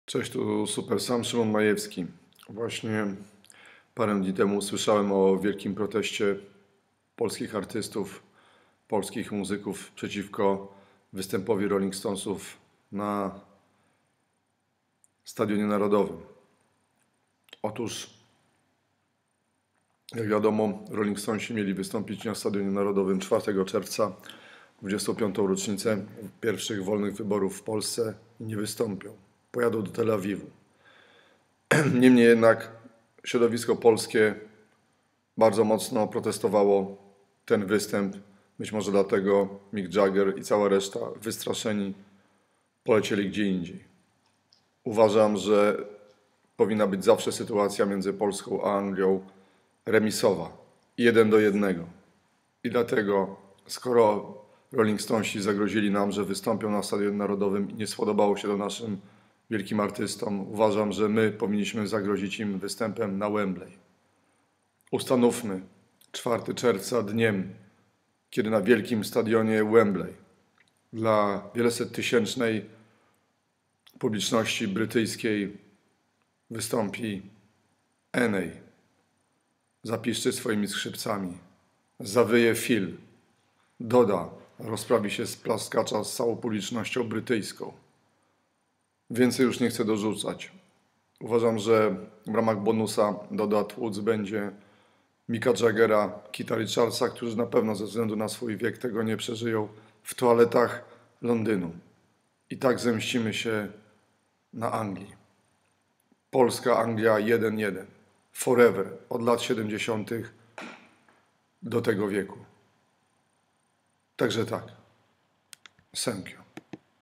Doda na Wembley!
Nam wciskają Rolling Stonesów?  Precz! My im też wciśnijmy. Polska - Anglia 1 : 1